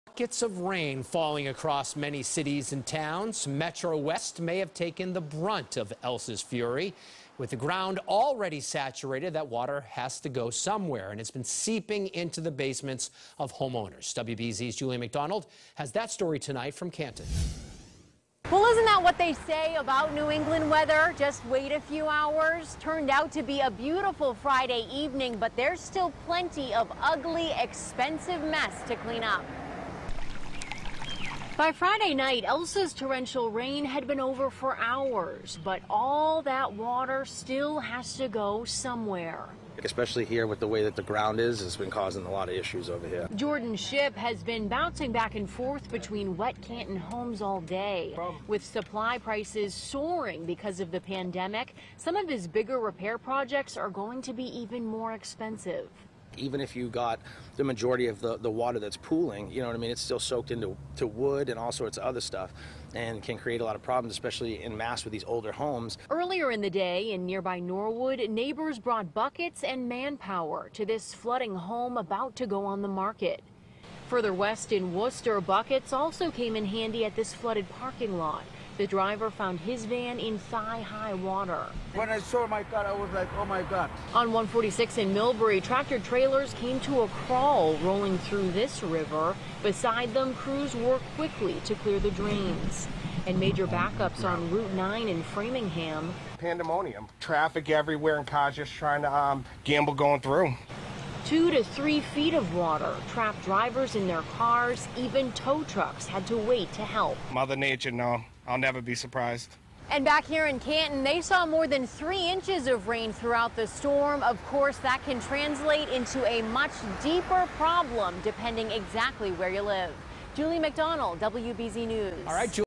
Tropical Storm Elsa Floods Roadways, Basements Across Mass.
WBZ-TV's Juli McDonald reports.